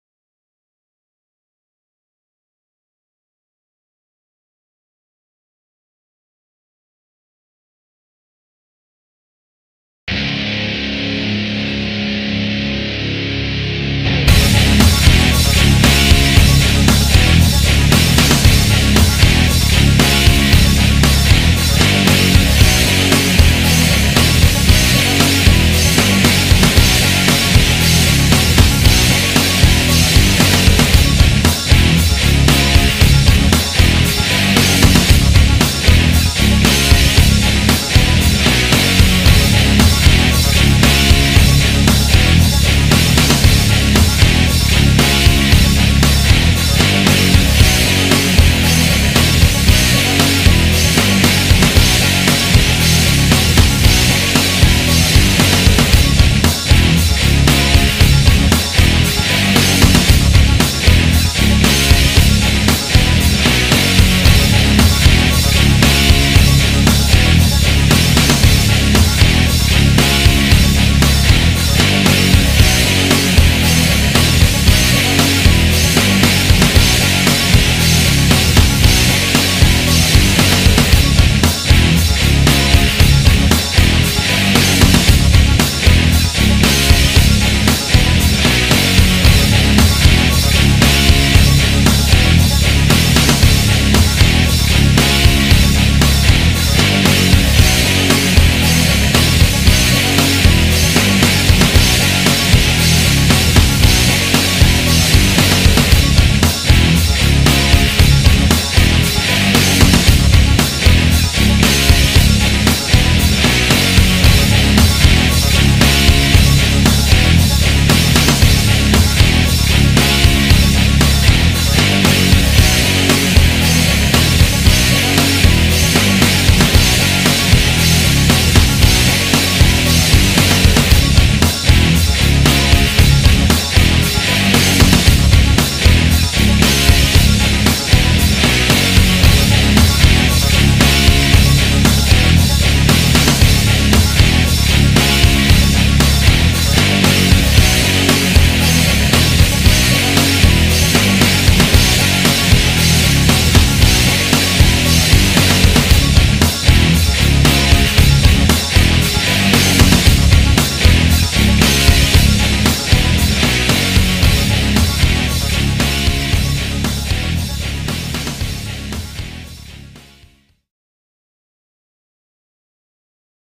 MLP (E.G) Characters W/Wrestling Themes: Bulk Biceps
Time to get Bulk some recognition here, so here's his 1st theme!